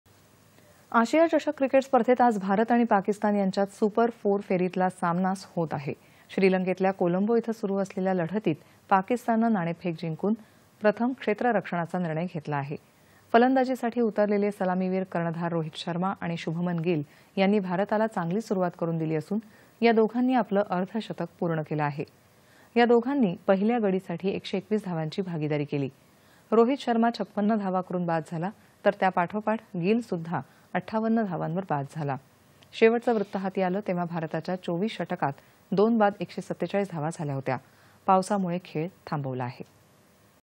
क्रिकेट -फलंदाजीसाठी उतरलेले सलामीवीर कर्णधार रोहित शर्मा आणि शुभमन गिल यांनी भारताला चांगली सुरूवात
दि.१०.०९.२०२३ :
 आशिया चषक क्रिकेट स्पर्धेत आज भारत आणि पाकिस्तान यांच्यात सुपर ४ फेरीतला सामना होत आहे. श्रीलंकेतल्या कोलंबो इथं सुरू असलेल्या लढतीत पाकिस्ताननं नाणेफेक जिंकून प्रथम क्षेत्ररक्षणाचा निर्णय घेतला आहे. फलंदाजीसाठी उतरलेले सलामीवीर कर्णधार रोहित शर्मा आणि शुभमन गिल यांनी भारताला चांगली सुरूवात करून दिली असून या दोघांनी आपलं अर्धशतक पूर्ण केलं आहे. या दोघांनी पहिल्या गडीसाठी १२१ धावांची भागिदारी केली. रोहित शर्मा ५६ धावा करून बाद झाला तर त्यापाठोपाठ गिलसुद्धा ५८ धावांवर बाद झाला. शेवटचं वृत्त हाती आलं तेव्हा भारताच्या २४ षटकत २ बाद १४७ धावा झाल्या होत्या.   पावसामुळे खेळ थांबवला.

8.30am | 11.00am | 1.00pm | 4.30pm | 7.00pm | 9.30pm